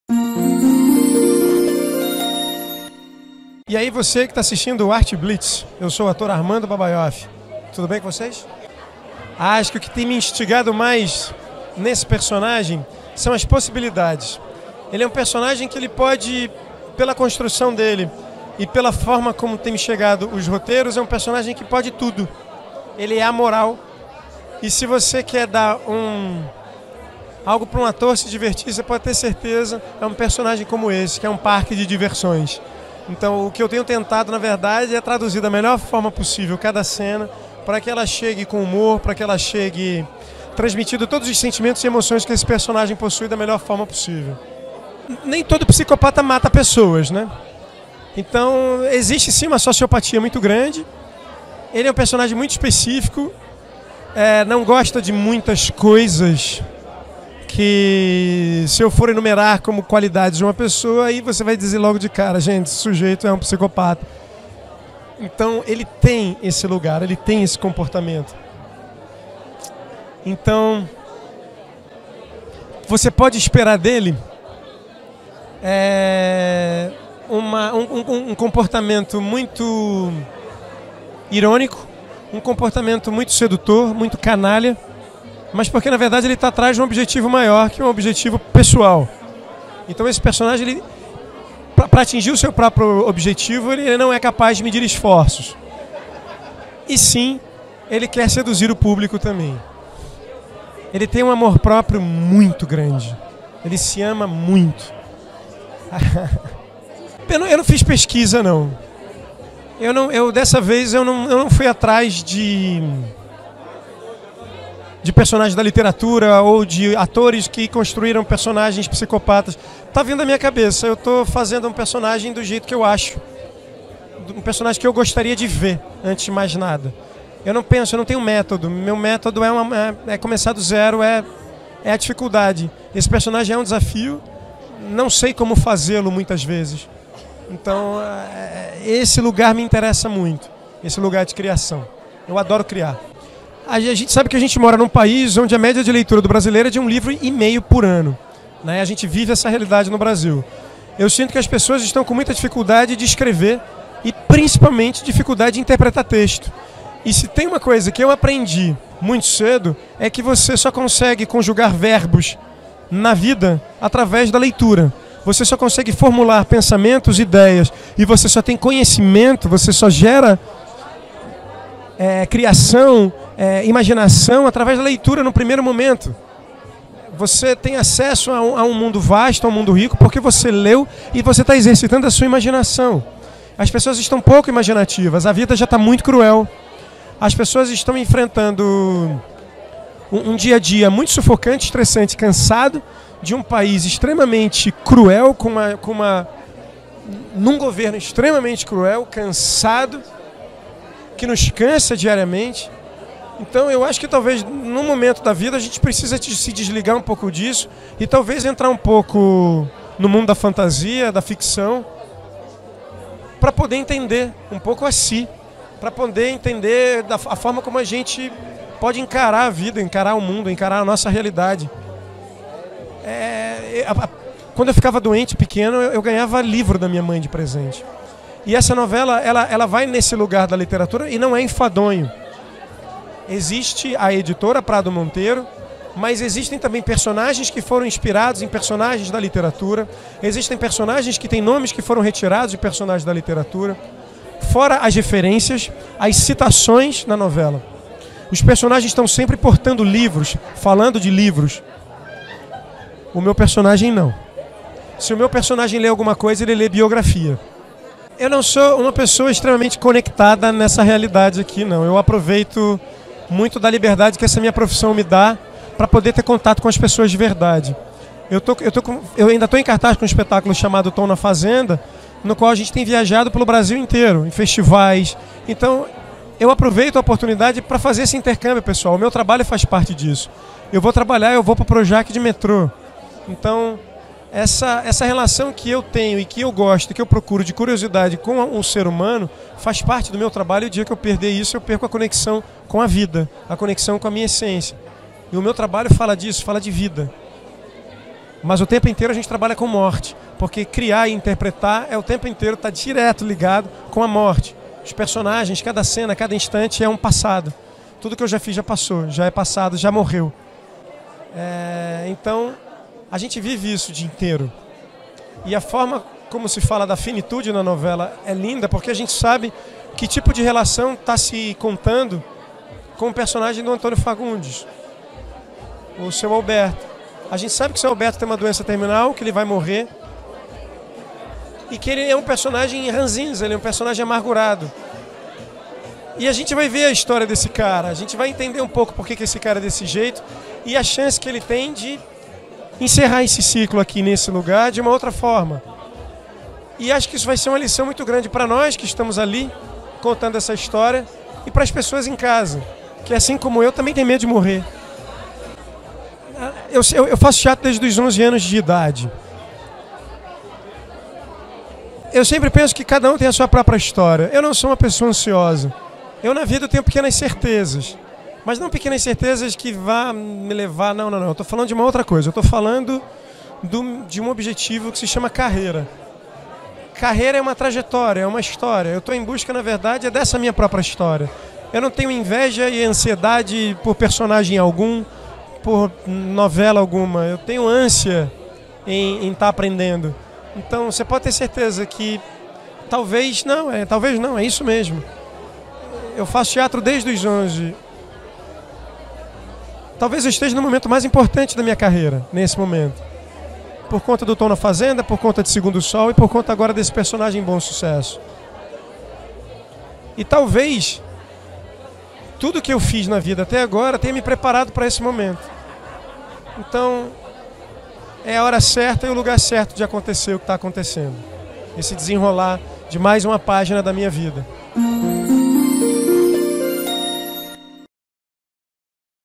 Armando Babaioff: “Diogo é um parque de diversões para qualquer ator”
Destaque como vilão da trama das 7 e premiado no teatro, ele reafirma suas certezas na arte